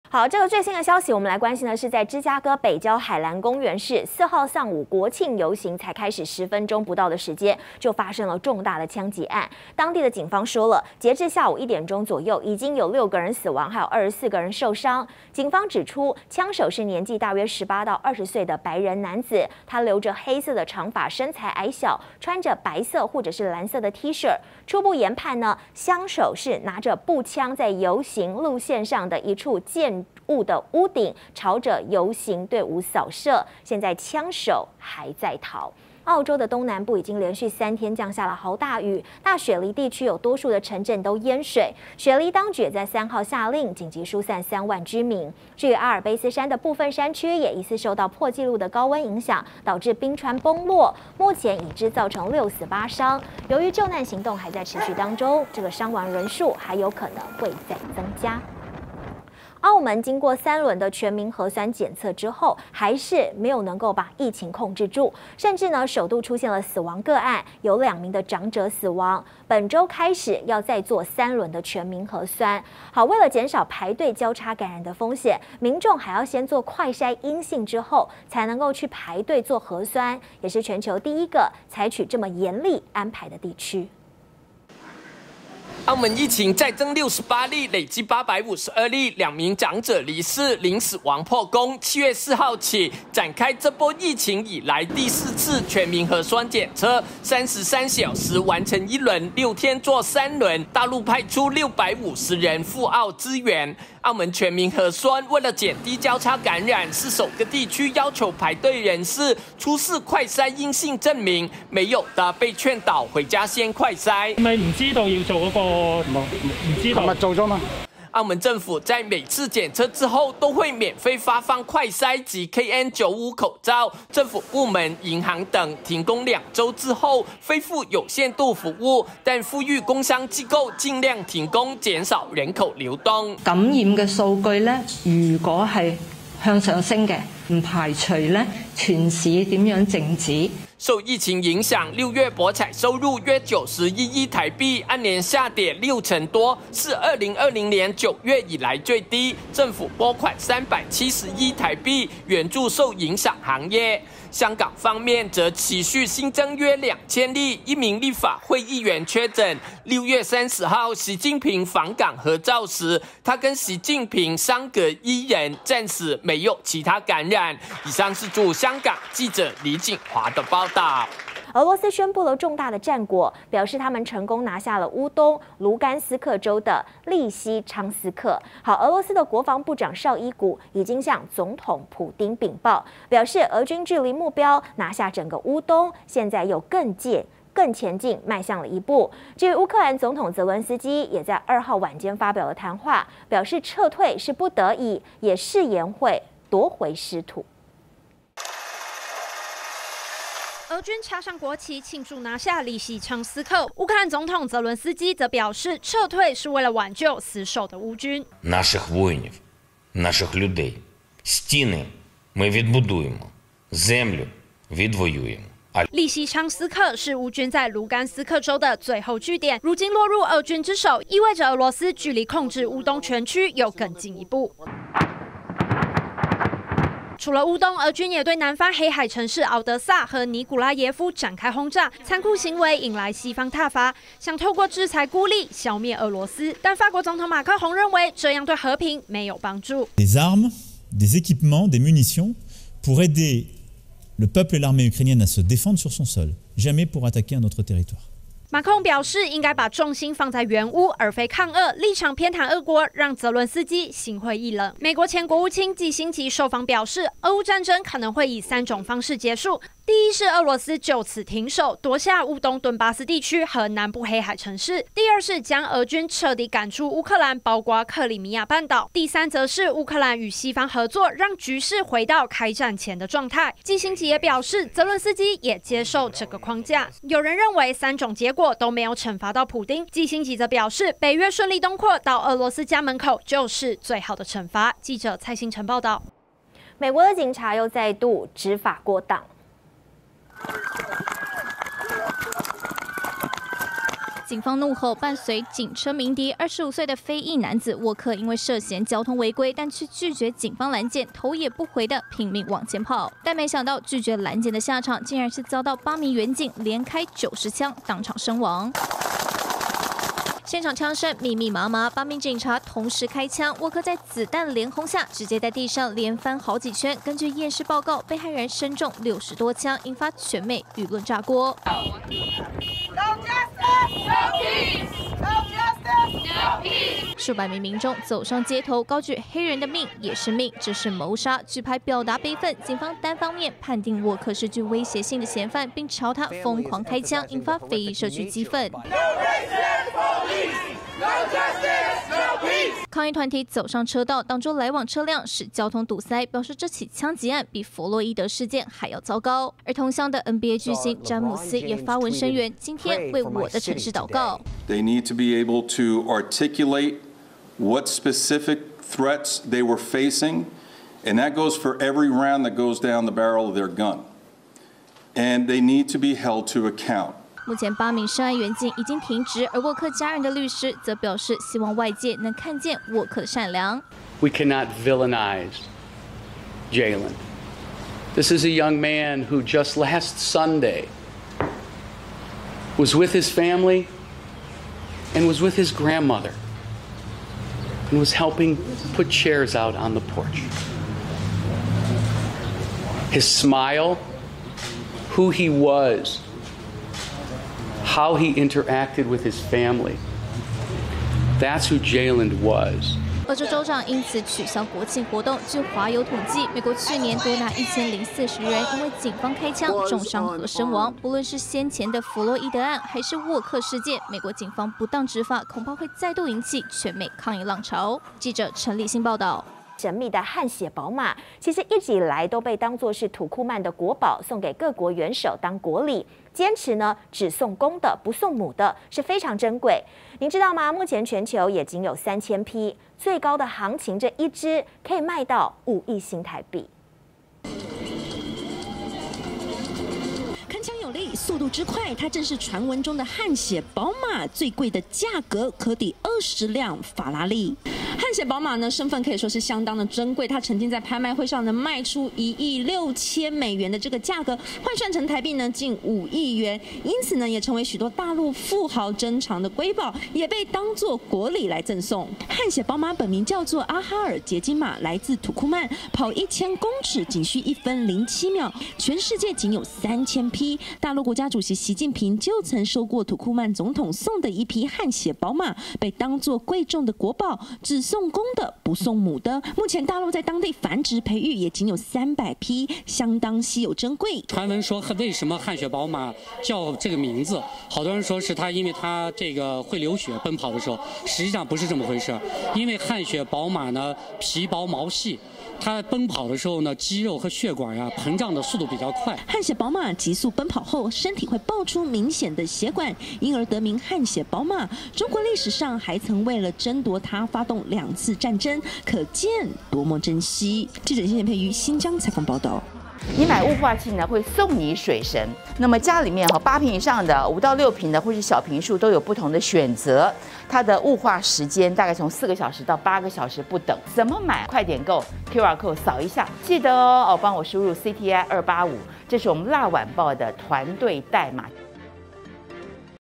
【全球現場日報】美非裔男遭警開槍斃命 NBA球星詹姆斯家鄉民眾連日抗議｜雪梨3天豪雨淹慘! 阿爾卑斯山冰川崩6死@全球大視野Global_Vision
00:00 芝加哥國慶遊行 6死24傷槍擊慘案 槍手在逃
00:37 雪梨3天豪雨淹慘! 阿爾卑斯山冰川崩6死
00:59 澳門6天3輪全民核酸 不排除推"全城靜止"
02:50 俄羅斯插旗烏東 澤倫斯基:誓死奪回失土
04:47 比佛洛伊德案還糟! 非裔男遭警轟90槍斃命